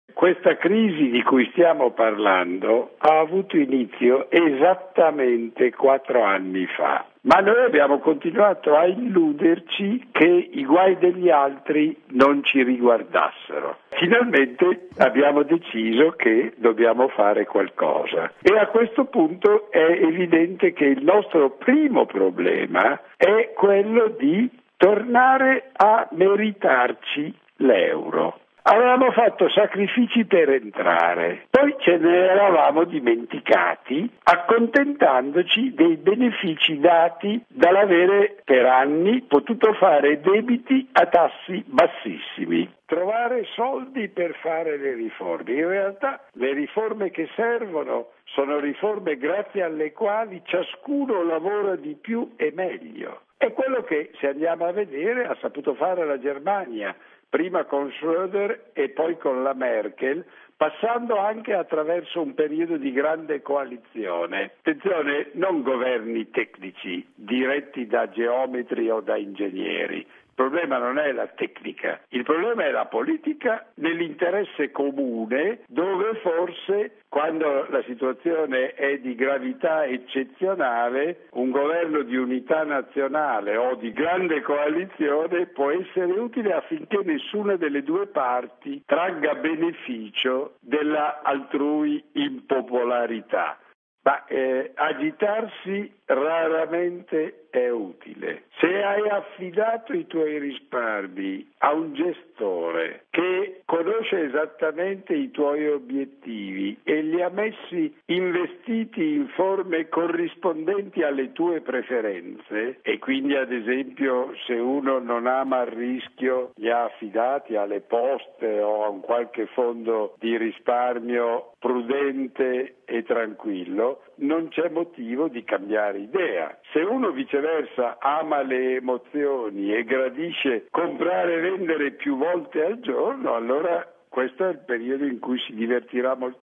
Berlusconi ha Negato la Crisi per 4 Anni ed Ora? SIAMO SULL'ORLO DEL FALLIMENTO!
Il commento di Giacomo Vaciago che insegna politica economica alla Cattolica di Milano.